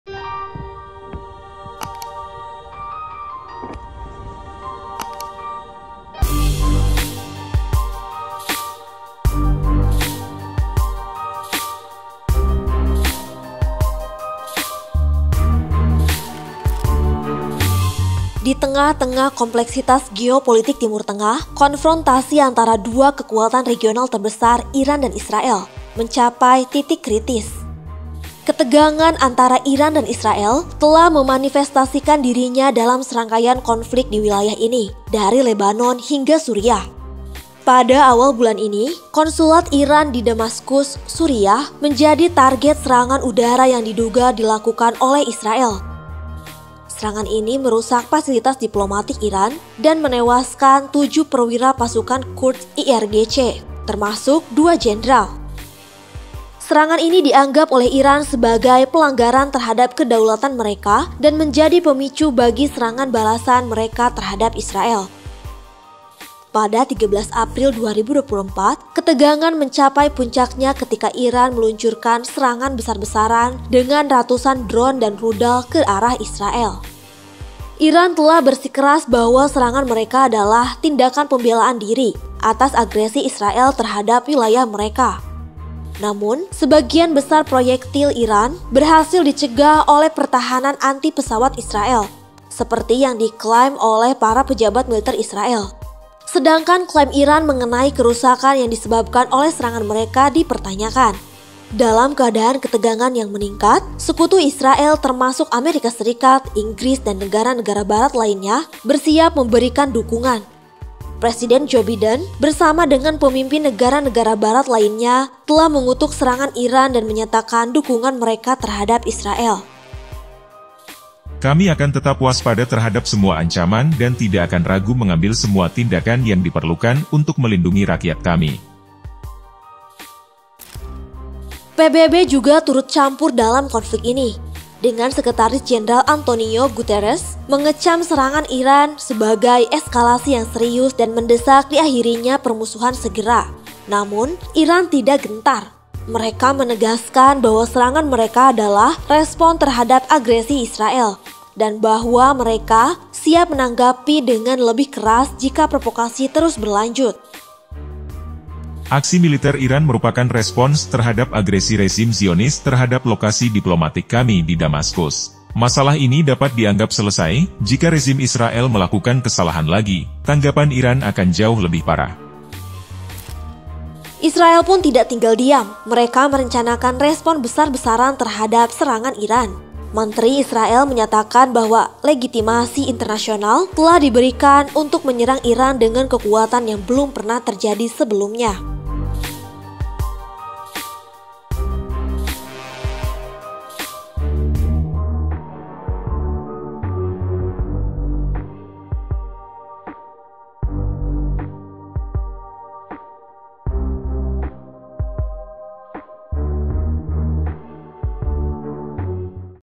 Eskalasi Konflik: Iran Melancarkan Serangan Balas Dendam Terhadap Israel | PRIANGAN.COM
Di tengah-tengah kompleksitas geopolitik Timur Tengah, konfrontasi antara dua kekuatan regional terbesar, Iran dan Israel, mencapai titik kritis. Ketegangan antara Iran dan Israel telah memanifestasikan dirinya dalam serangkaian konflik di wilayah ini, dari Lebanon hingga Suriah. Pada awal bulan ini, konsulat Iran di Damaskus, Suriah, menjadi target serangan udara yang diduga dilakukan oleh Israel.